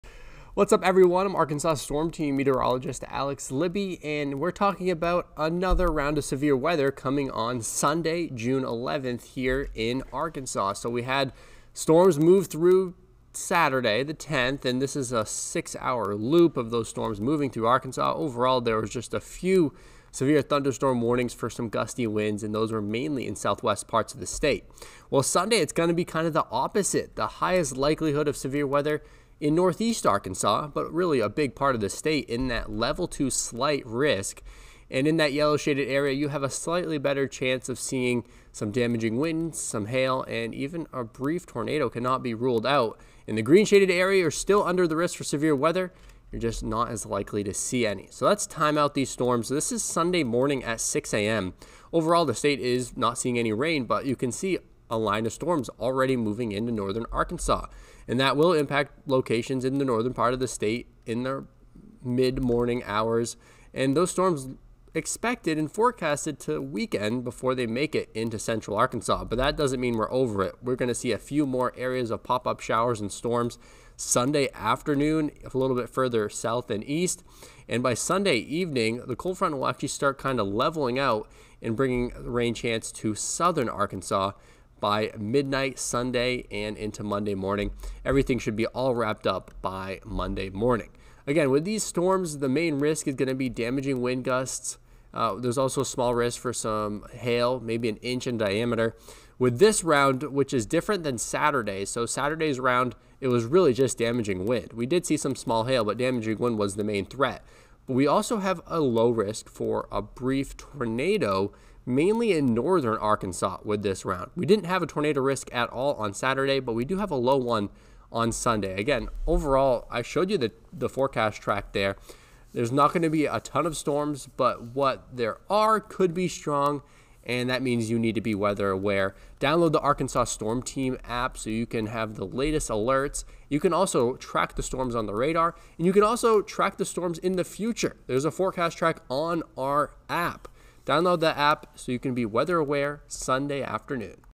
Another round of Severe thunderstorms to impact Arkansas Sunday June 11th 2023
Meteorologist Alex Libby breaks down Arkansas' next round of severe weather.